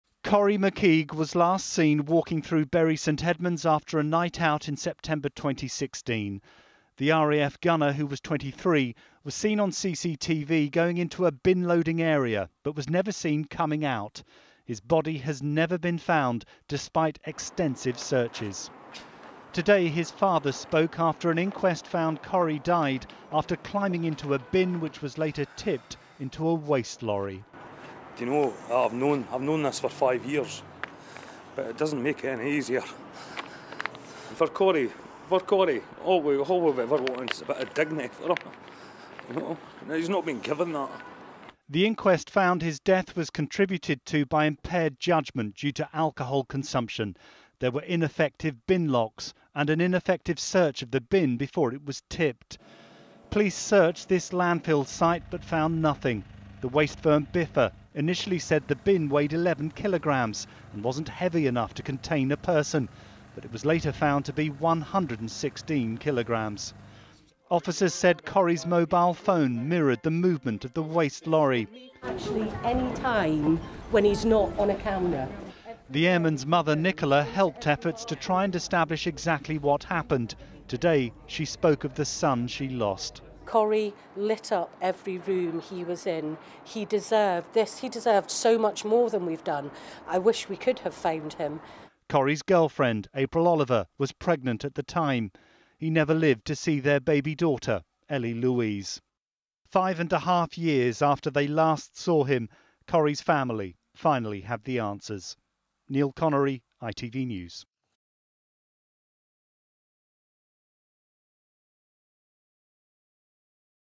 Missing airman Corrie McKeague was crushed to death in bin lorry, inquest rules | ITV News
RAF gunner Corrie McKeague, who vanished on a night out in 2016, died after he went into a bin which was tipped into a waste lorry, an inquest has concluded.

The airman, of Dunfermline, Fife, was 23 when he disappeared in the early hours of 24 September in Bury St Edmunds, Suffolk.

On Tuesday, an inquest jury in Ipswich recorded in a narrative conclusion that Mr McKeague was in a bin that was tipped into a waste lorry.